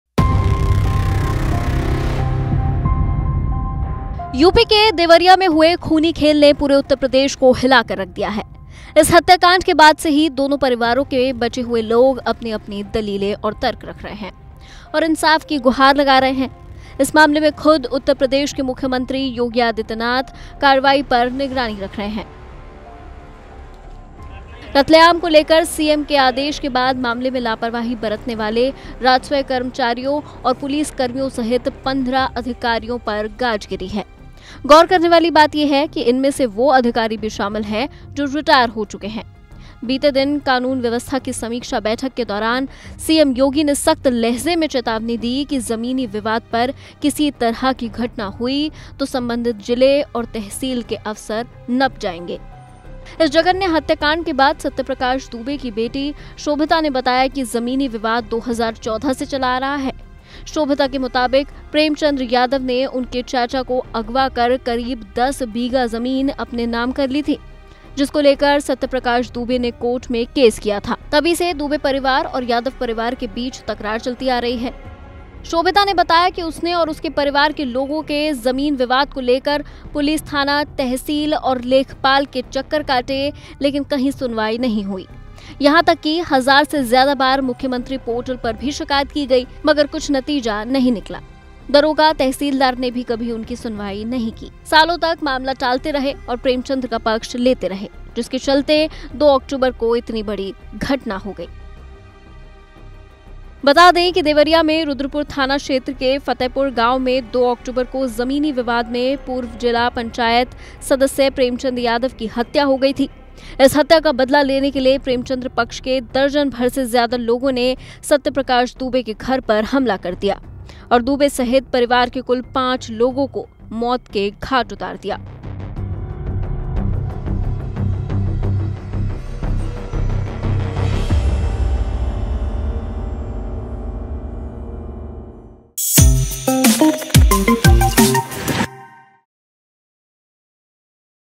Deoria Case: एक्शन में Yogi सरकार, बेटियों के बयान के बाद नपेंगे अधिकारी ! abplive
News: Deoria Case: एक्शन में Yogi सरकार, बेटियों के बयान के बाद नपेंगे अधिकारी ! abplive